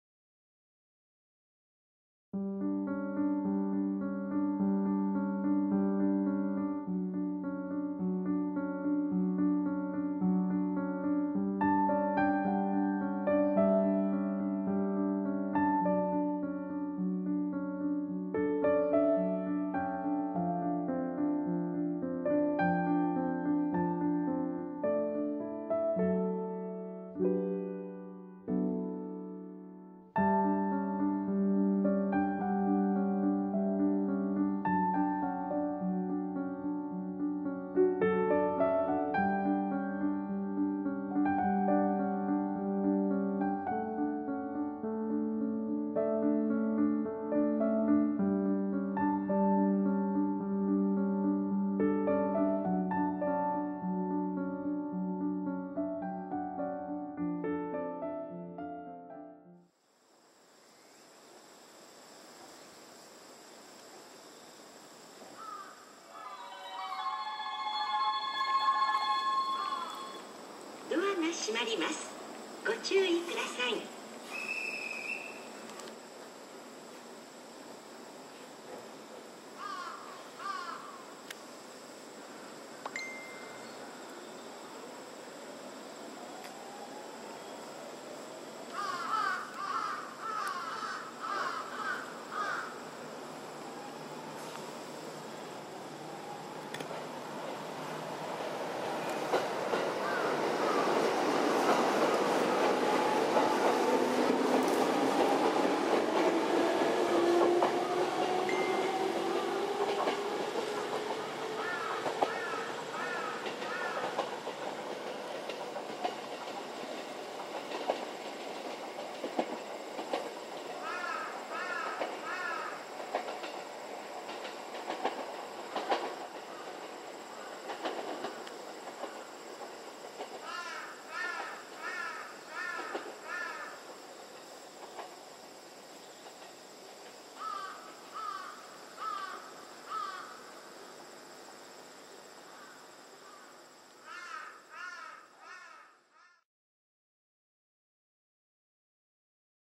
JR相馬駅（2015.08.22）
JR相馬駅。
入場券を購入して中に入り、Panasonic LUMIX DMC-TZ20で撮影。